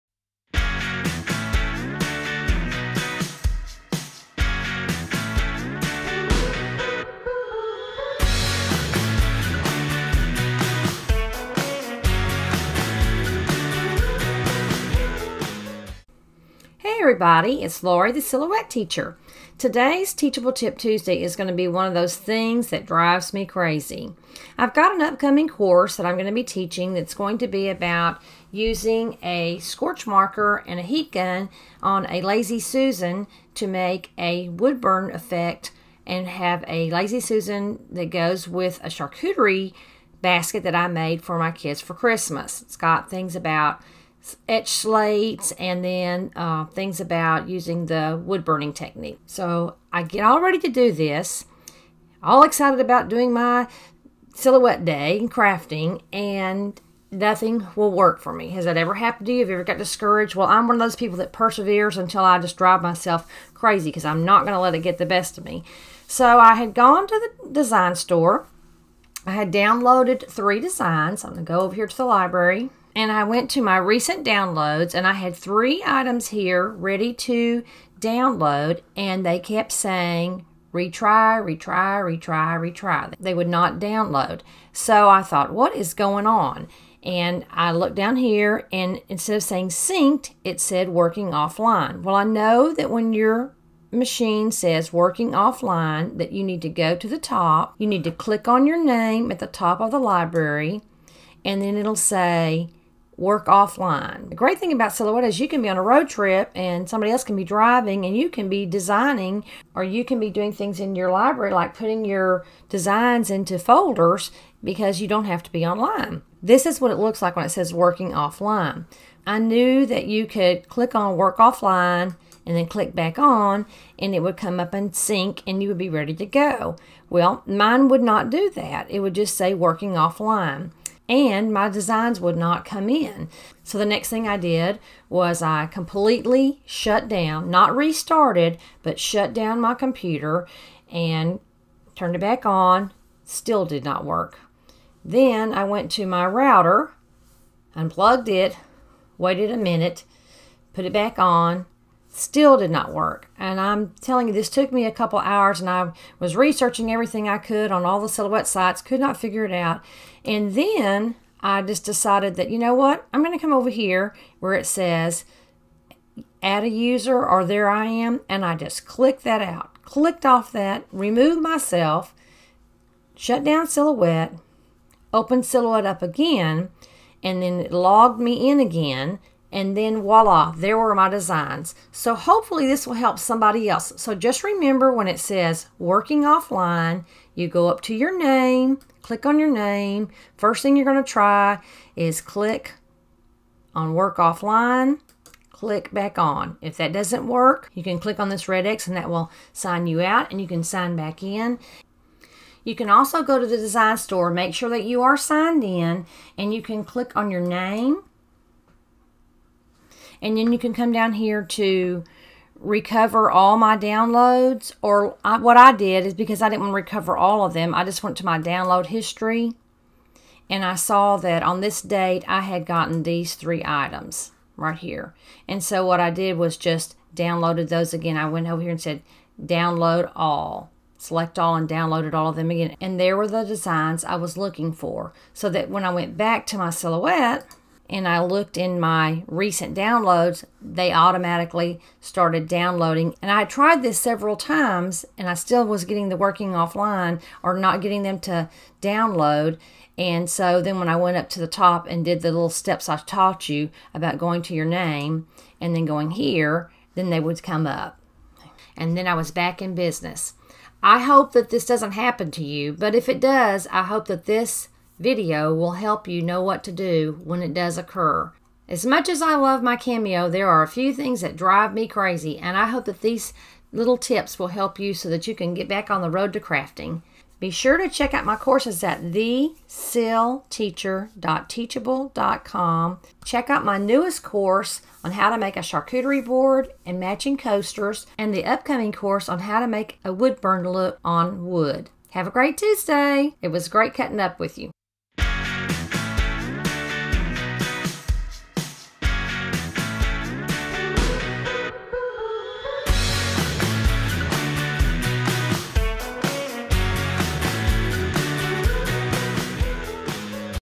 Silhouette Studio Cameo - My Design Store Designs Won't Load Into My Silhouette Library
Do you have things that drive you crazy when you are using your Silhouette device?  Hopefully, this video will help you when your purchased designs won't download into your Silhouette library.  Soon you will back in business crafting again!!!  Can't wait to start cuttin' up with YOU!